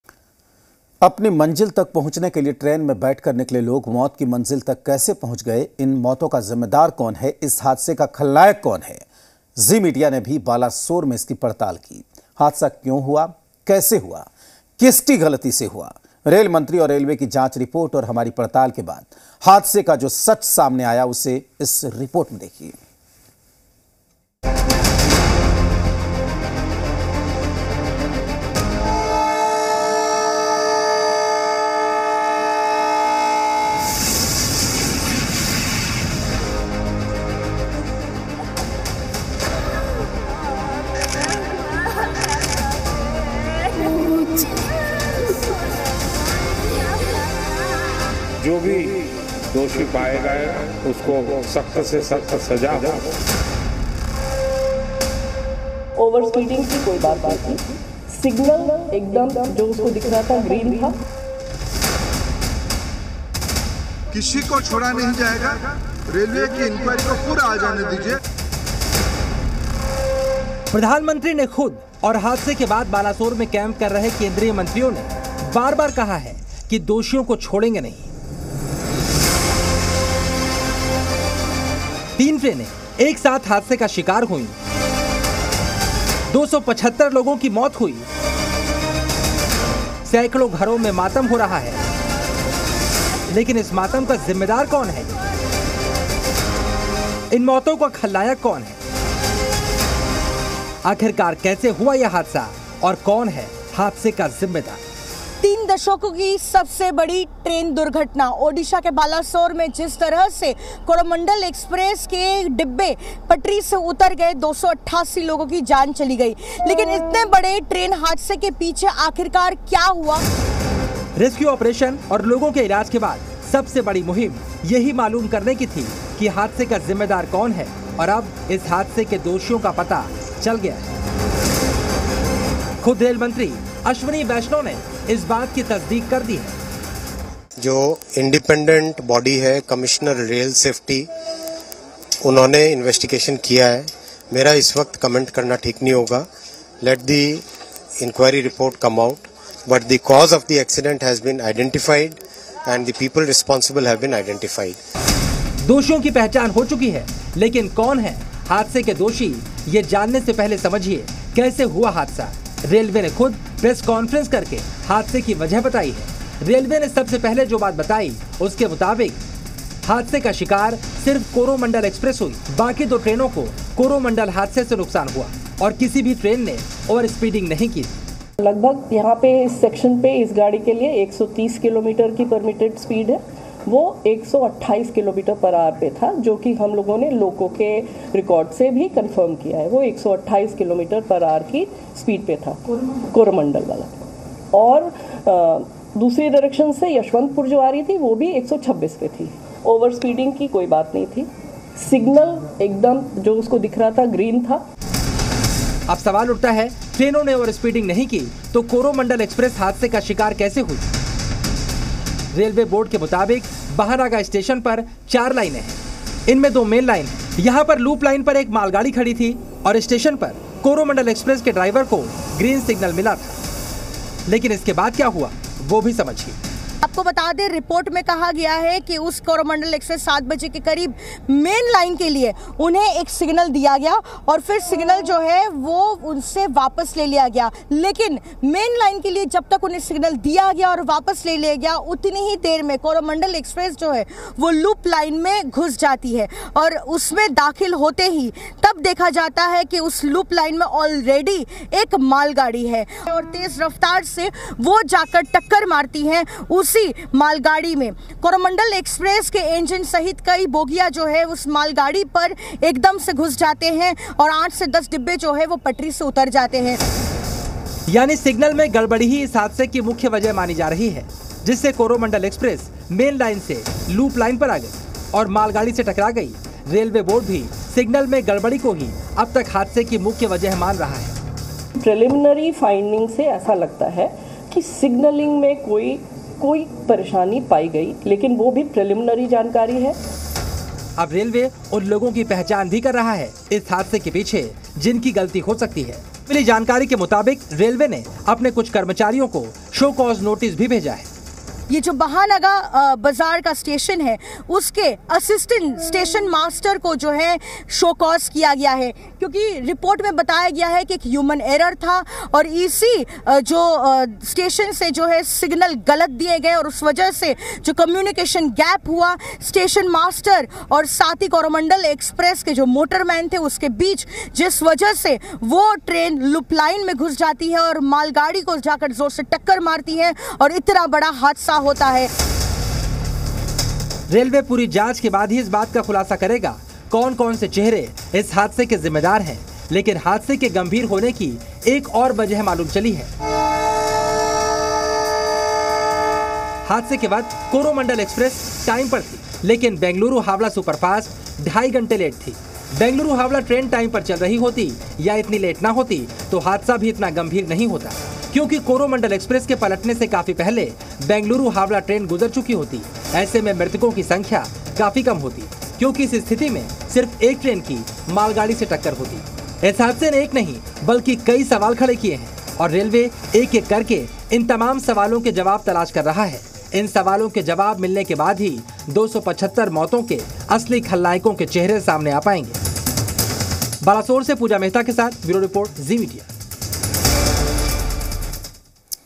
Odisha Train Accident: रेलवे सिग्नल कैसे बना मौत का सिग्नल ? | Balasore Train Accident
अपनी मंजिल तक (Odisha Train Accident) पहुंचने के लिए ट्रेन में बैठकर निकले लोग मौत की मंजिल तक कैसे पहुंच गए .इन मौतों का जिम्मेदार कौन है...इस हादसे का खलनायक कौन है...ज़ी मीडिया ने भी बालासोर (Balasore Train Accident) में इसकी पड़ताल की. हादसा क्यों हुआ...कैसे हुआ...किसकी गलती से हुआ...रेल मंत्री और रेलवे की जांच रिपोर्ट और हमारी पड़ताल के बाद हादसे का जो सच सामने आया है...उसे इस रिपोर्ट में देखिए...